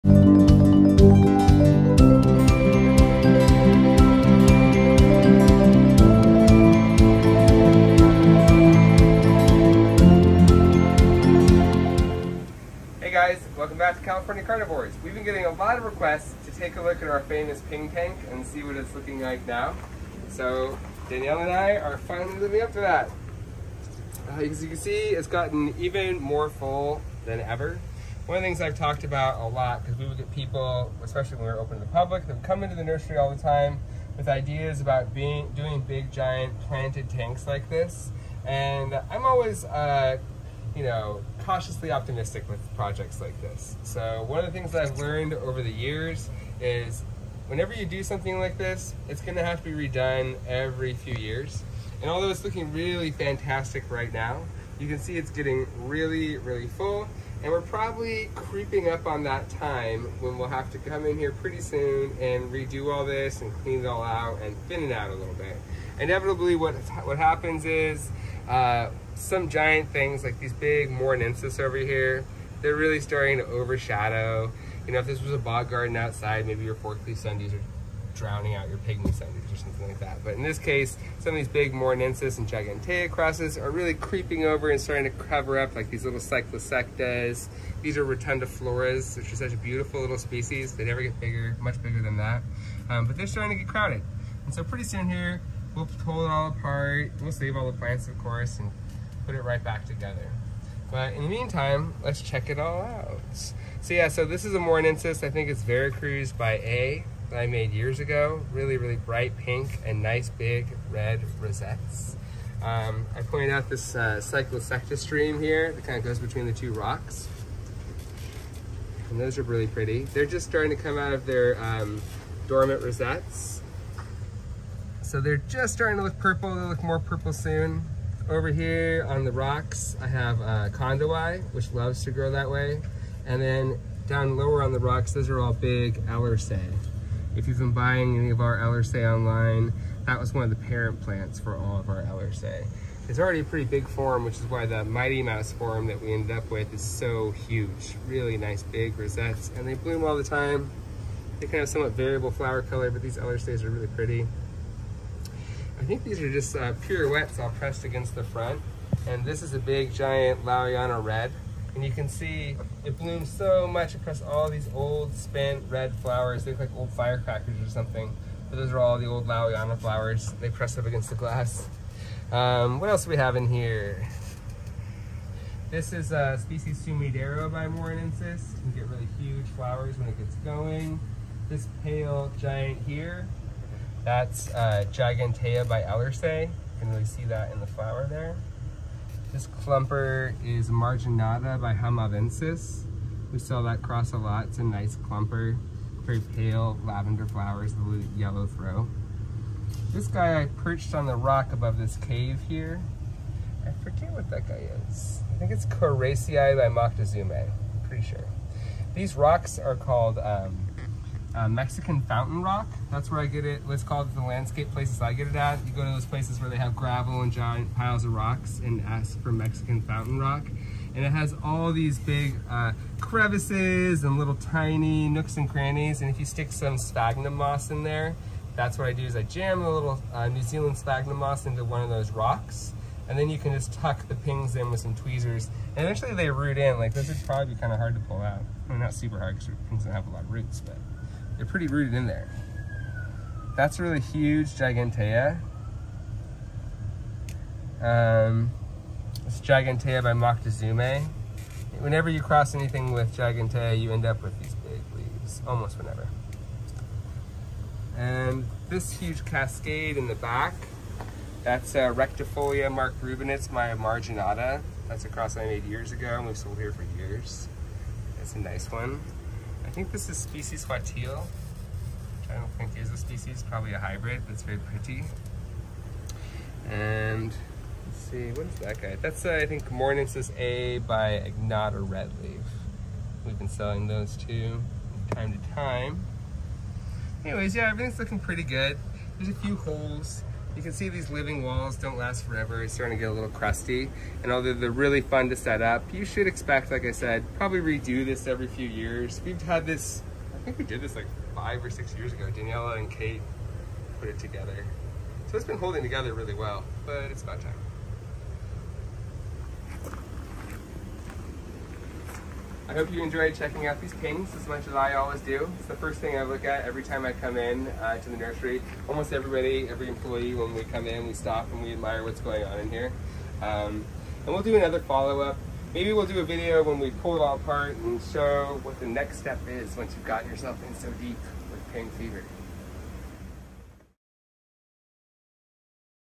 Giant Carnivorous Plant Terrarium
We planted this Pinguicula tank a few years ago and let’s take a look at it, now that all the plants have grown in and several natural cycles have come through.